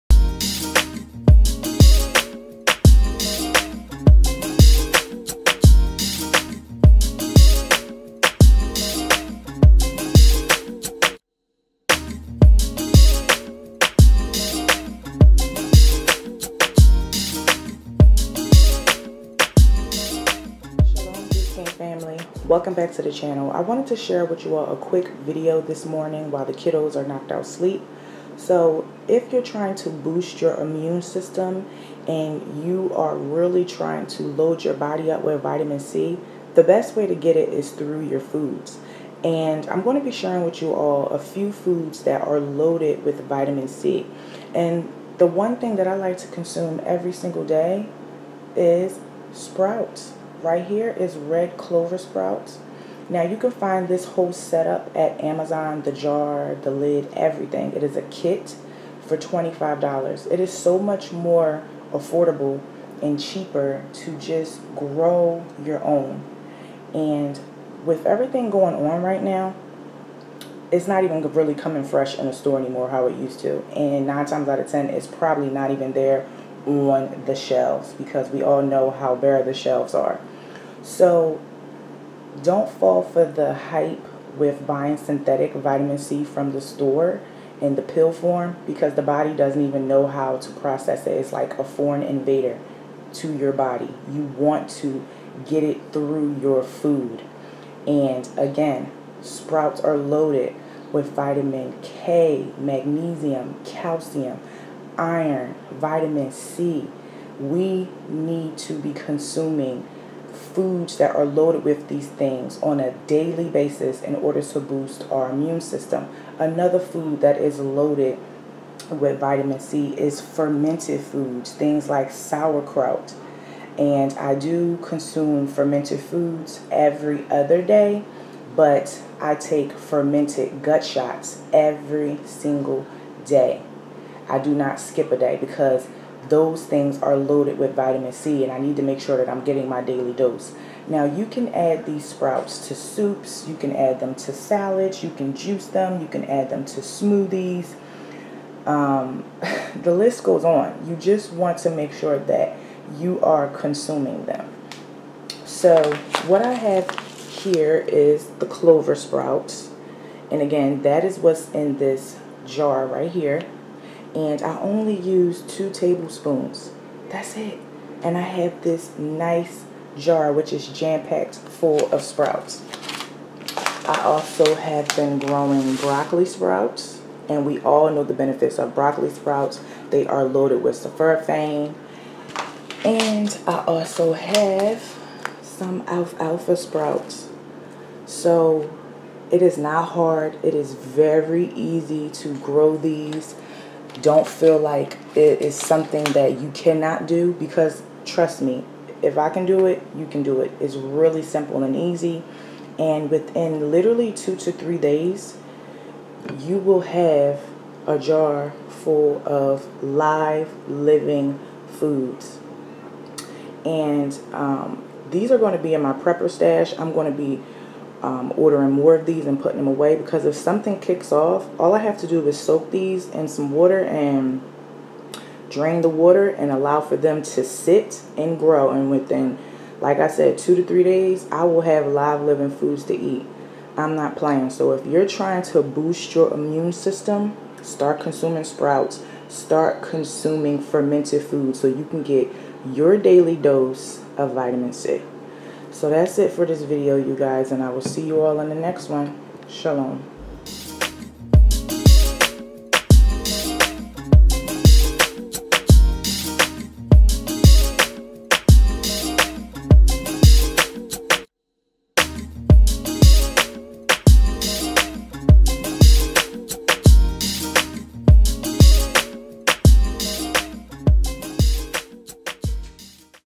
HOW TO INCREASE YOUR VITAMIN C LEVELS WITH SPROUTS!!
Shalom Juice Game Family,

Today I Wanted To Share With You All A Quick Video On How You Can Naturally Increase Your Vitamin C Levels With Sprouts A lot Of People Believe That Buying Over The Counter Vitamin C From Walmart Or CVS Is Best Way To Increase Your Vitamin C Levels When This Information Is Not True You Are Causing More Harm Then Good To Your Body The Best Way To Boost Your Immune System Is To Eat The Foods That Are High In Vitamin C If You Want To Take a Supplement Then Buy A Plant Based (food based) Supplement To Increase Your Vitamin C Levels You Can Also Drink Herbal Tea Which Is Also Loaded With Vitamin C Too Remember To Always Do Your Research So That Way You Can Have A Clear Understanding On What To Take To Improve Your Health & If You All Like Seeing Videos Like This Hit That Like Button & Subscribe!!